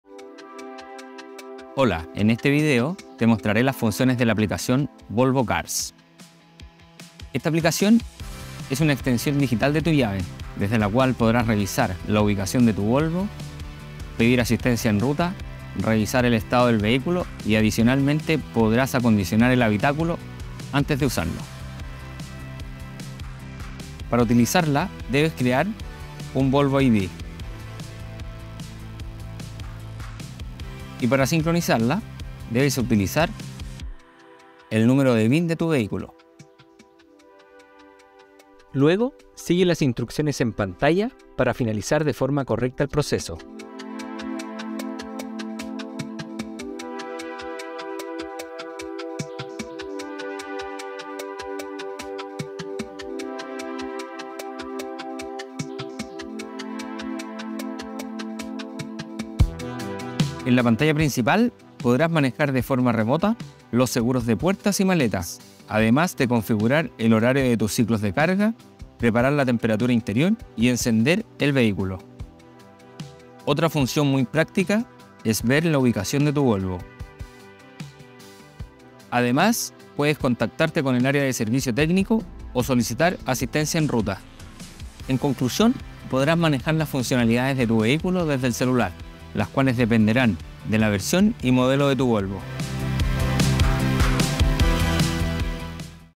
Volvo Advice: Aprende a usar las funciones de la app Volvo Cars
Descubre junto a nuestro experto las funciones de la App.